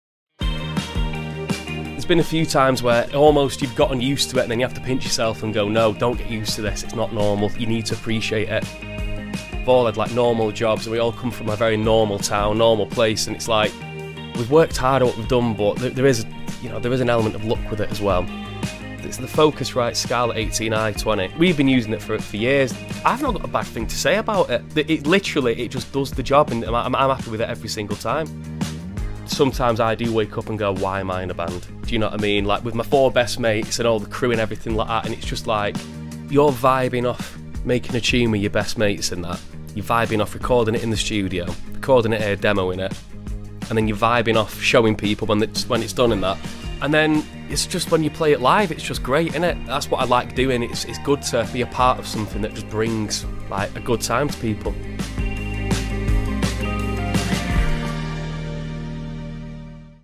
Blossoms - N’arrêtez jamais de créer // Focusrite (la Boite Noire du Musicien)
Nous célébrons les 10 ans de la première Scarlett de Focusrite. En 10 ans, la Scarlett a accompagné plus de 4 millions de créateurs passionnés et s’est imposée comme l’interface audio de référence.